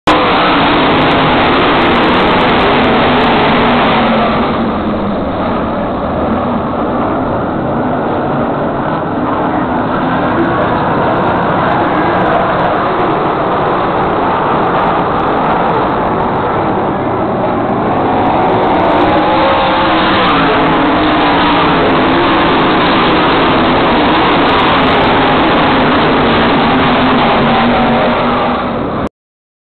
Picture 843
Bob Weikert memorial--Port Royal Speedway--4/26/2014 (Greg Hodnett lst, 2/Dale Blaney, 3rd Brent Marks,4/ Shane Stewart